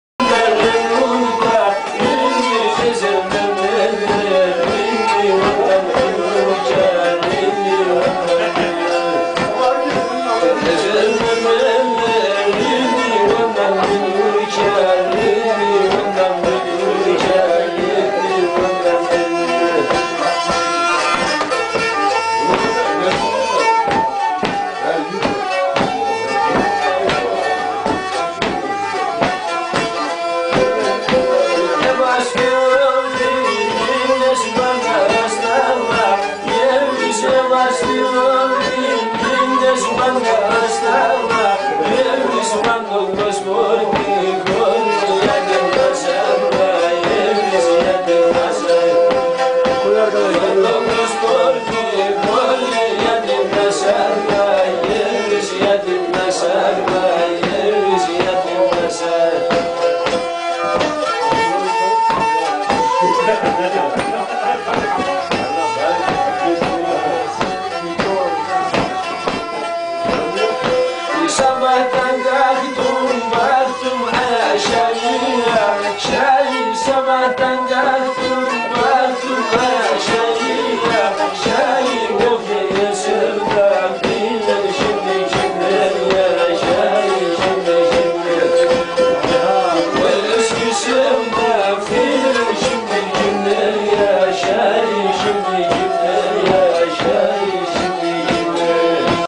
Harika Karadeniz Havasi
Kemence Kaval bir arada olursa böyle güzel ses cikar.